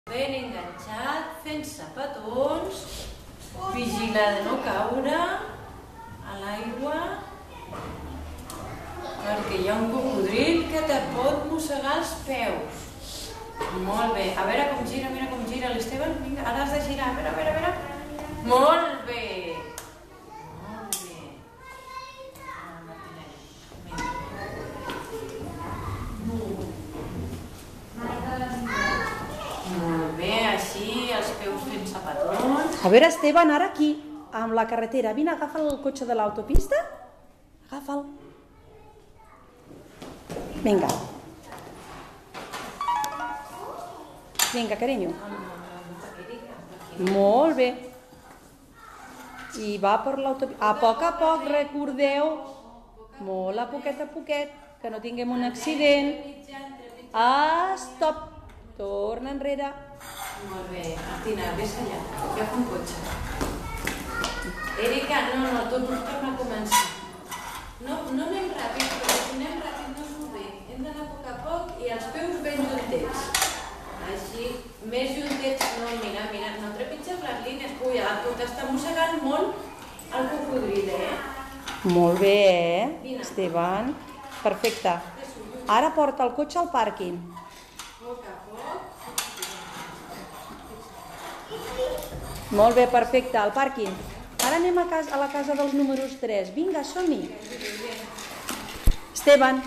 El número 3 .
Mireu com els nens de P3B aprenen a fer el número 3.